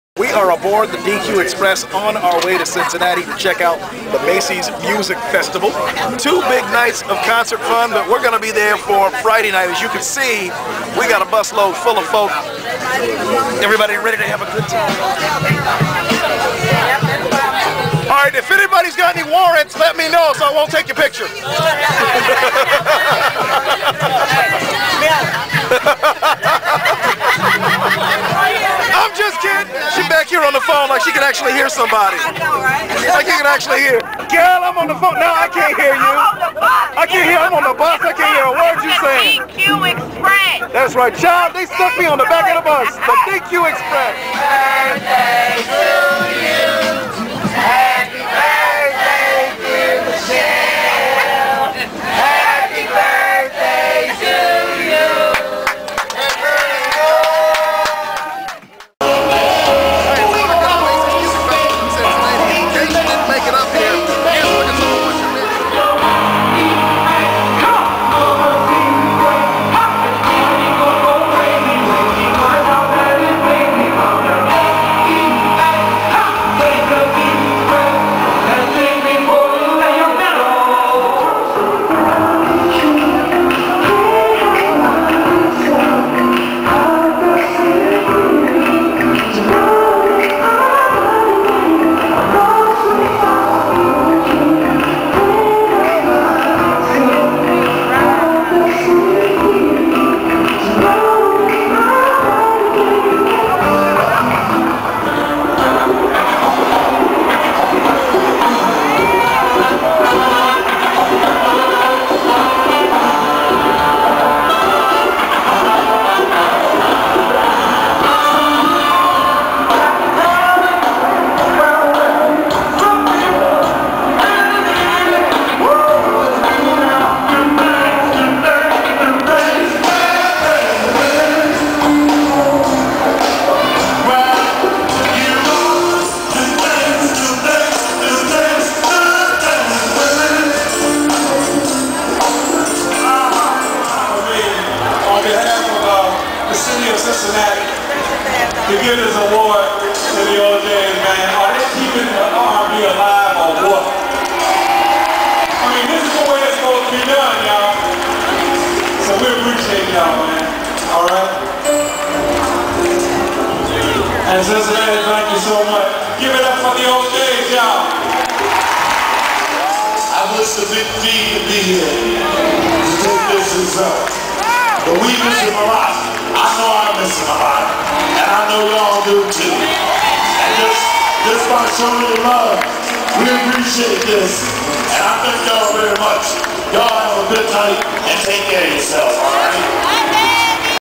The D.Q. Express To The Macy's Music Festival
We teamed up with Dairy Queen to take a bus load of listener's to one of the hottest concerts of the summer.